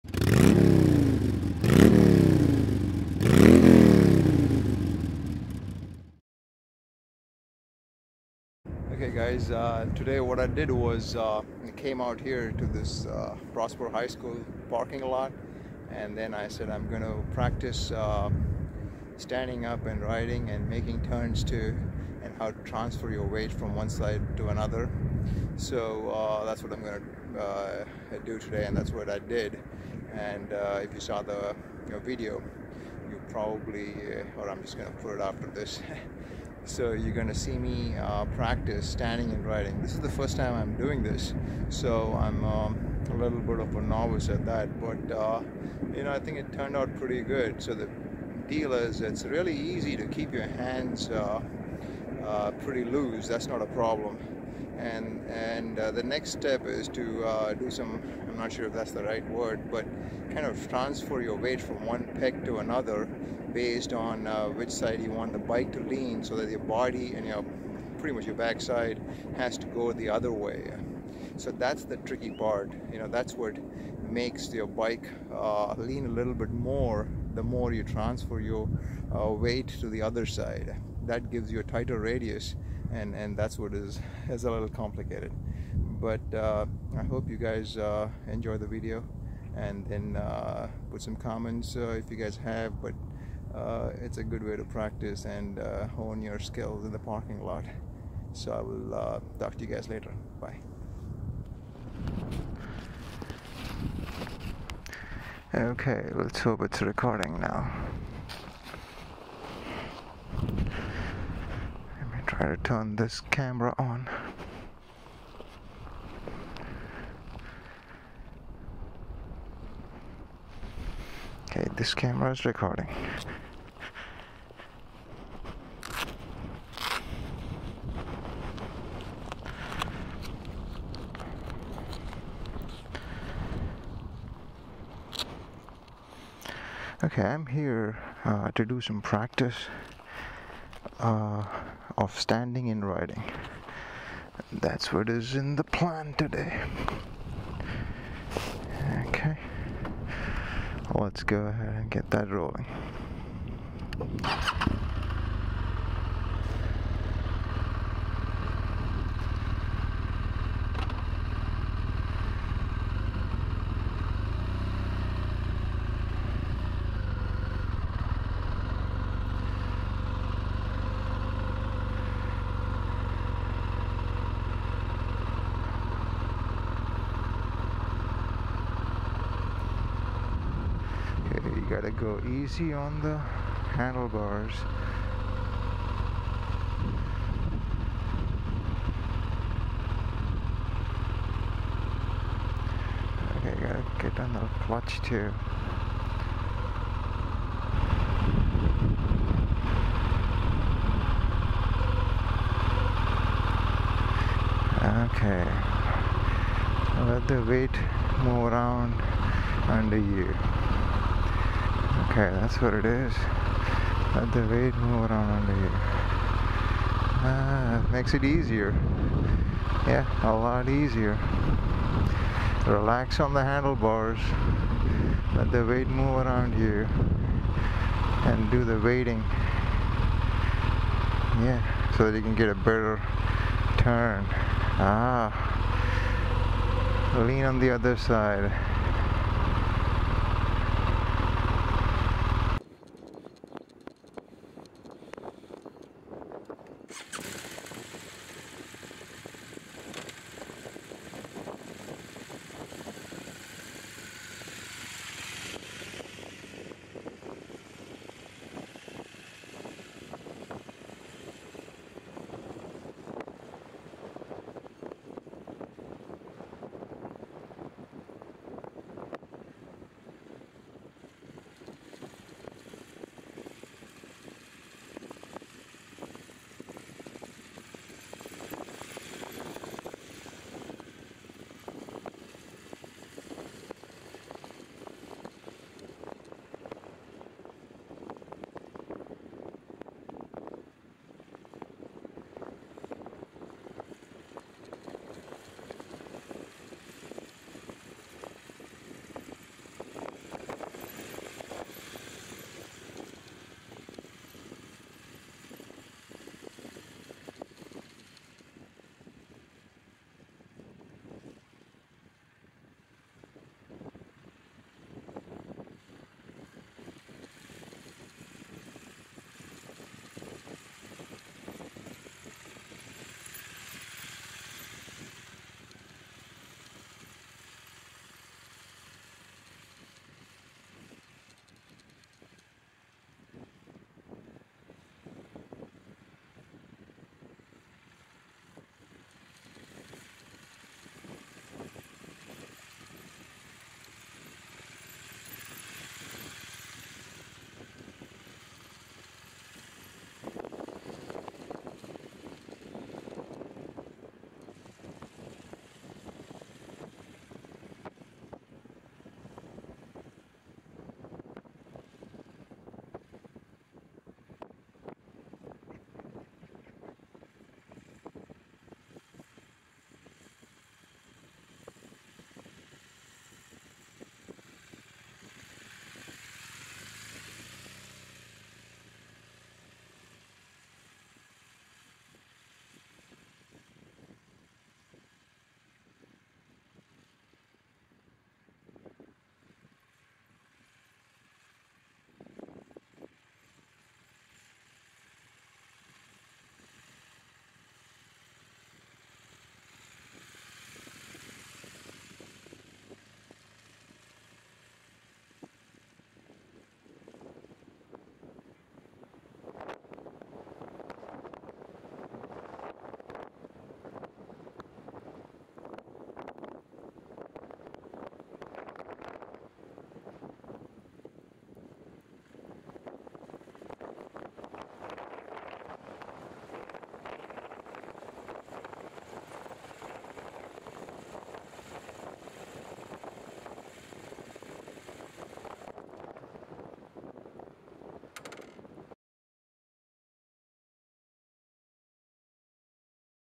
Standing and Riding on BMW 1200 GSA! Parking lot practice
I am standing and riding on my GSA for the first time, practicing in the parking lot.